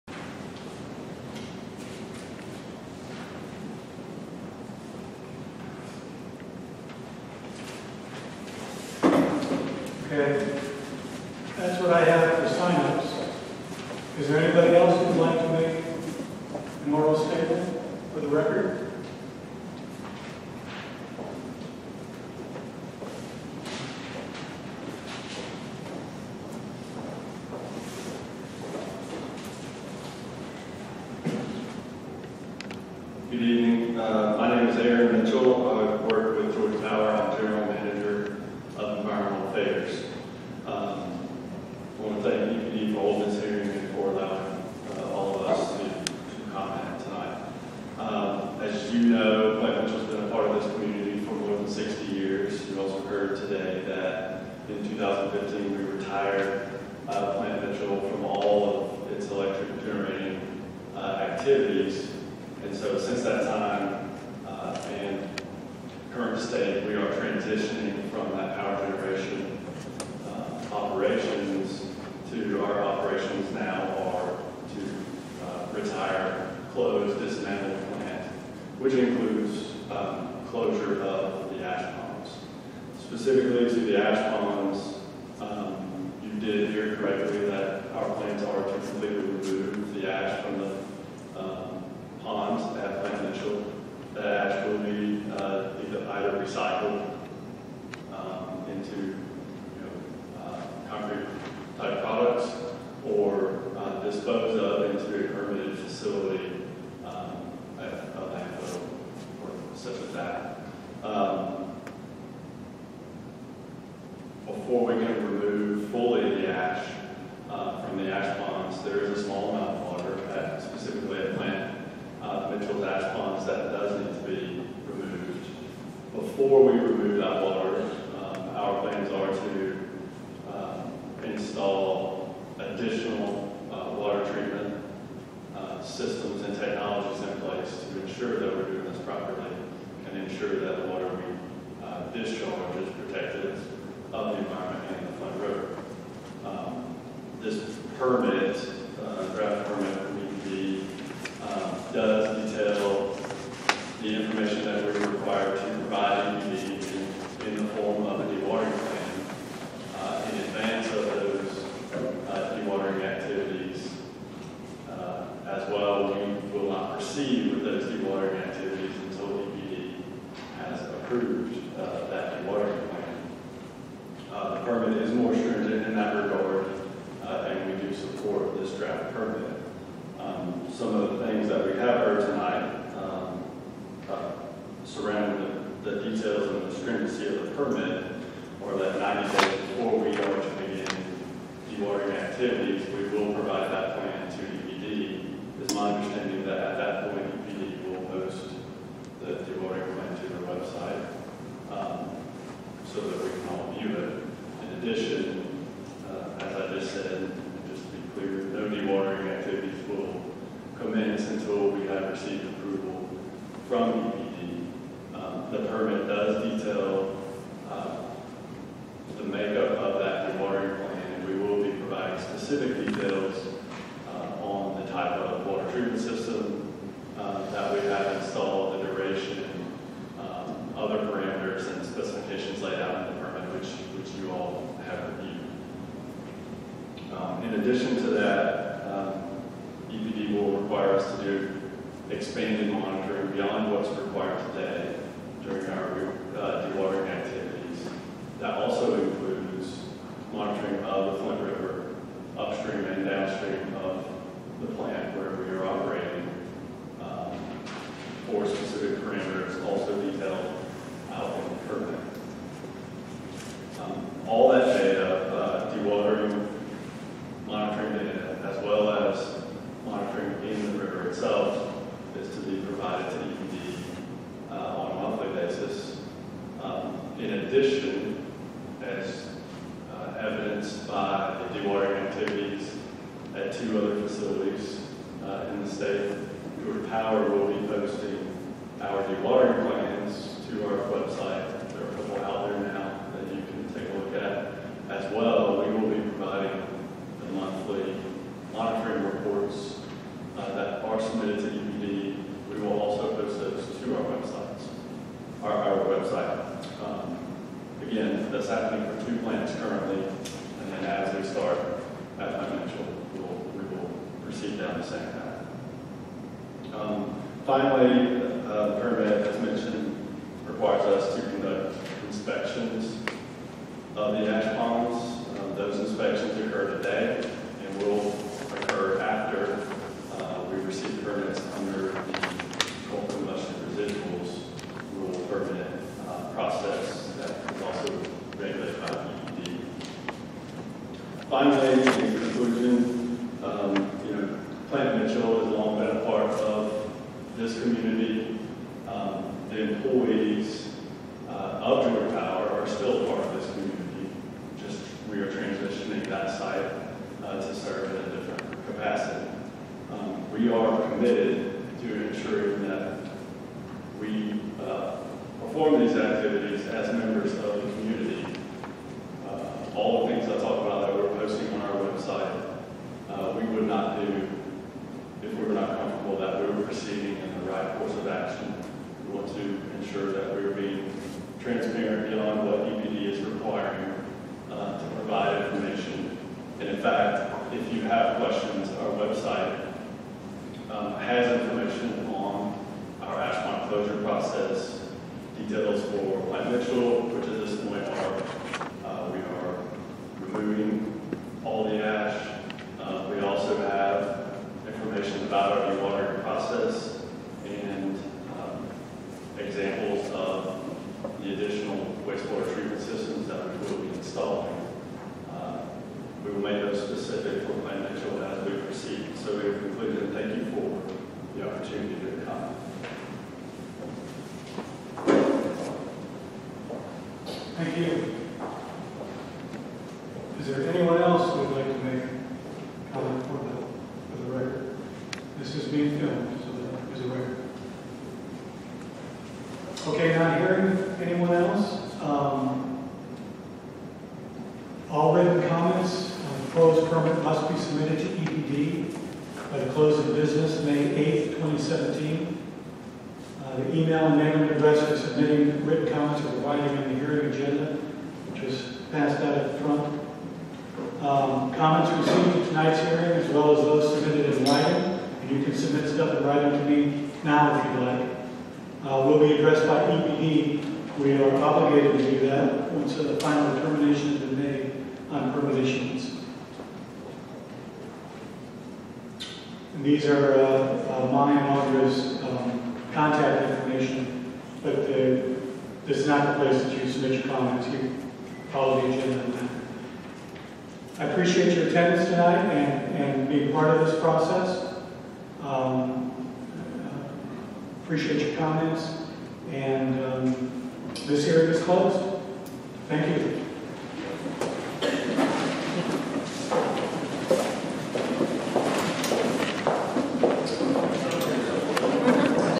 Georgia Power Representative Aaron Mitchell @ EPD Permit Hearing - 2017-05-04
Georgia EPD Public Hearing on Plant Mitchell Coal Ash Pond Permitting - Albany Georgia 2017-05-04